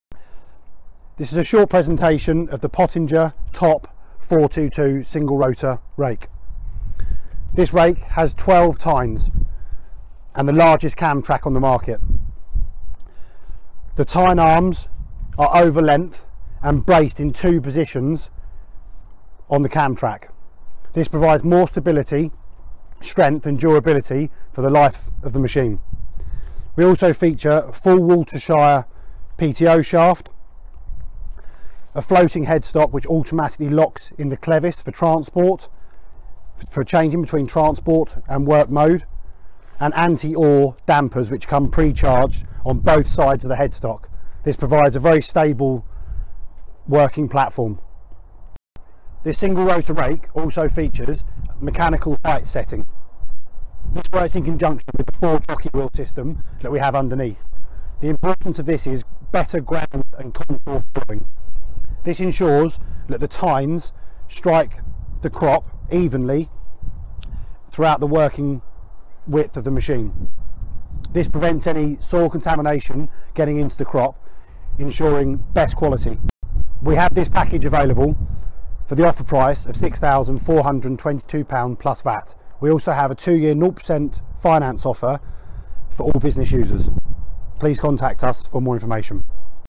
Burden Bros Agri walkaround of a Pottinger Top 422 single rotor rake
Group Sales Manager Harry Durrant showing us around a stock Pottinger Top 422 single rotor rake.